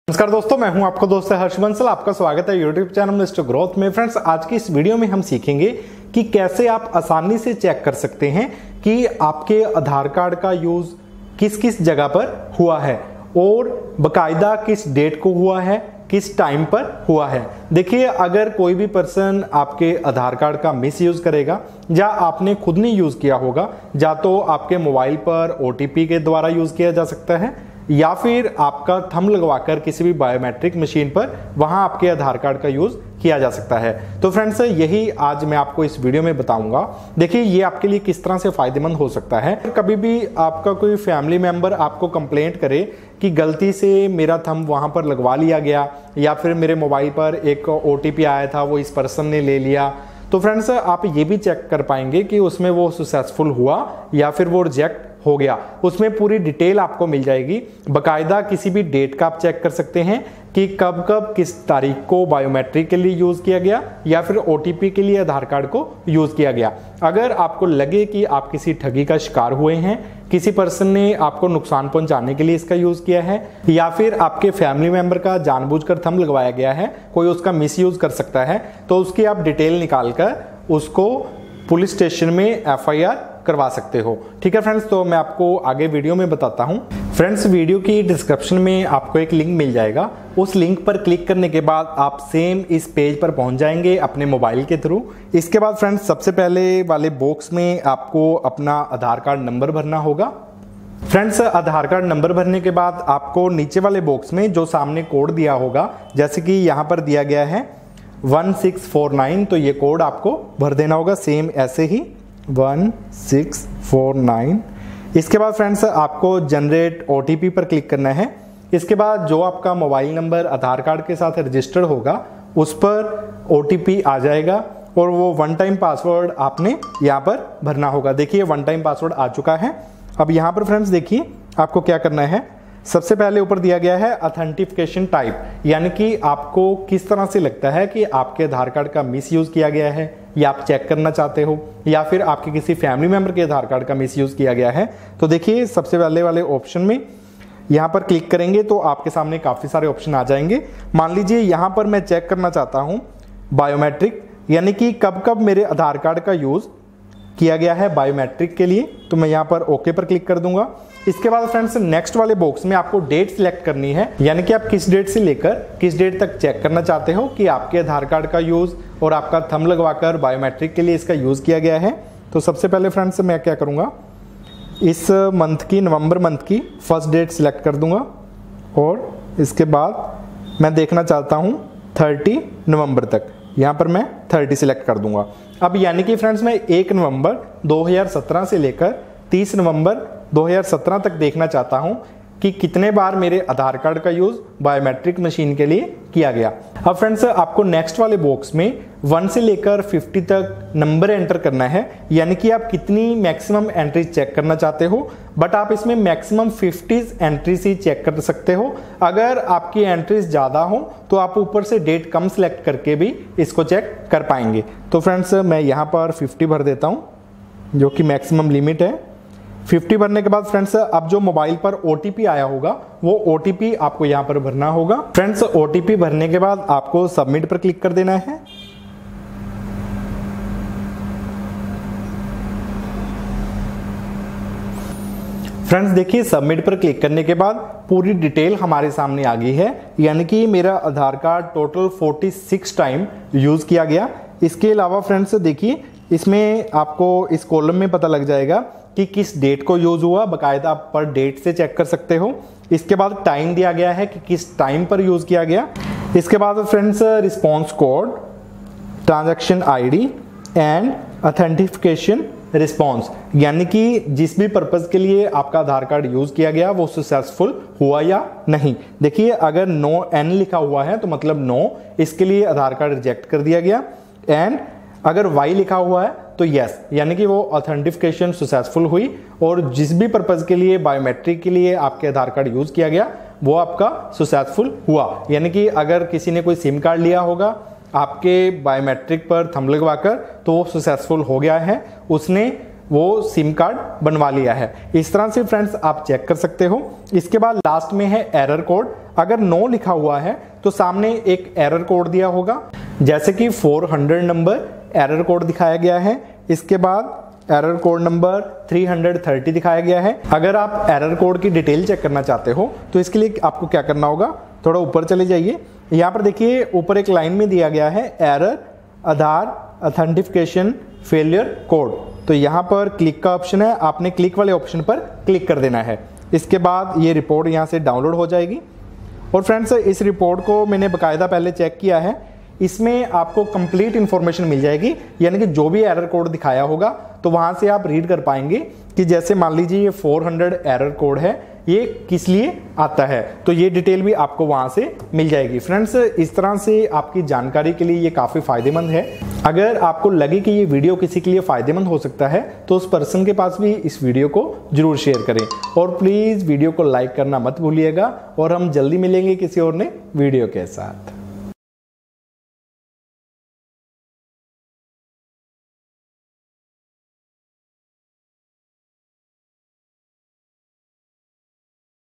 Aadhaar | आधार कार्ड का प्रयोग Biomatric और OTP के लिए कब किया गया पूरी डिटेल चेक करें | Mr.Growth🙂
Friends is video main aapk sath aadhaar card k using ka status check karna ka method share kia hai Aadhaar | आधार कार्ड का प्रयोग Biomatric और OTP के लिए कब किया गया पूरी डिटेल चेक करें | Mr.Growth🙂